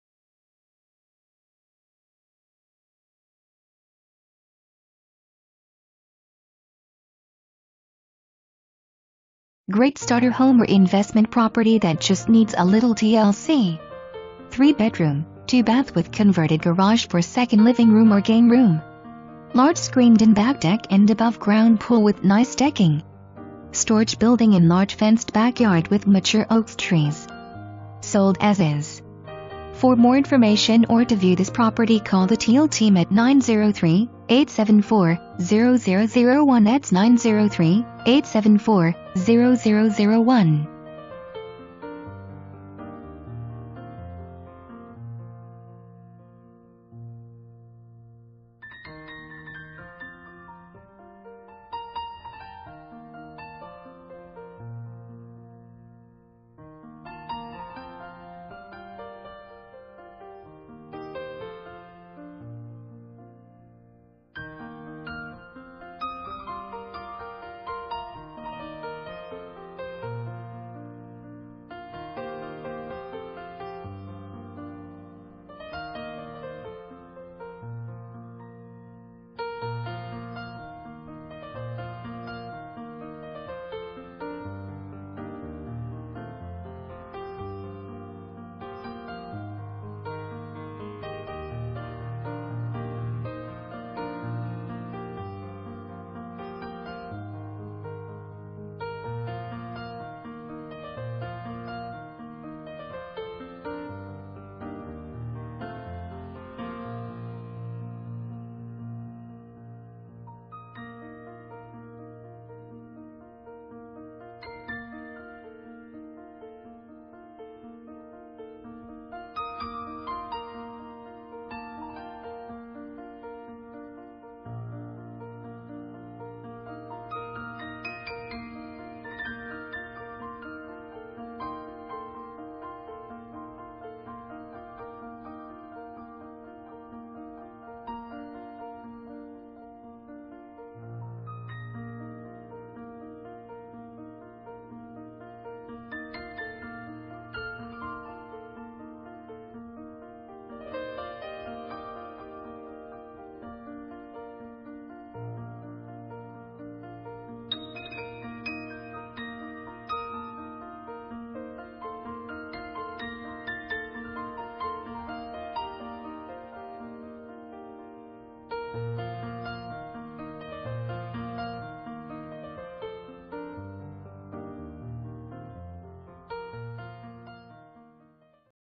2220 N Beaton St
3 bed, 2.0 bath, 1,741 SF, MLS# 13477478

Great starter home or investment property that just needs a little TLC! 3 bdrm, 2 bath with converted garage for second living room or game room. Large screened in back deck and above ground pool with nice decking. Storage building and large fenced backyard with mature oaks trees. Sold as is.

Presented By:
Julie Teel, RE/MAX of Corsicana & RE/MAX Lakeside Dreams